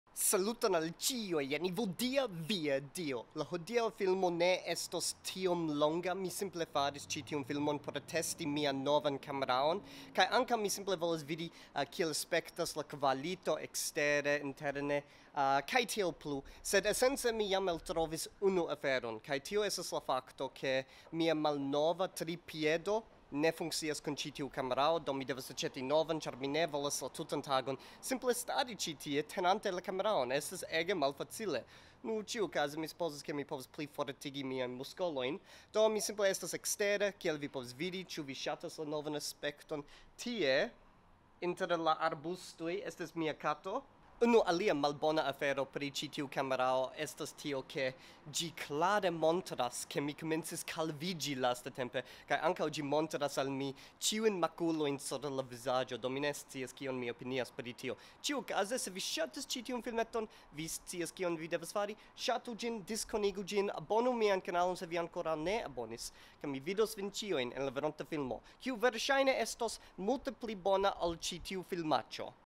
Mi provas mian novan kameraon
En ĉi tiu filmeto, mi provas mian novan kameraon. Mi jam malkovris, ke mi bezonas tenilon (aŭ tripiedon) kaj eĉ stabilon por ĝi.

Myriad (Miriado): MEx6VEmx8t72yP1eRofRLKc7dwtqVhKR7n

Monero: 49KTTNjgFs7VEnSwRj4YfaPRnpPjt1e19Dp7hpczoY7XDZYha4og4LM7PTtRFx1sH9gYxXx5ENpqCB8A3w3y27dn5YVmneK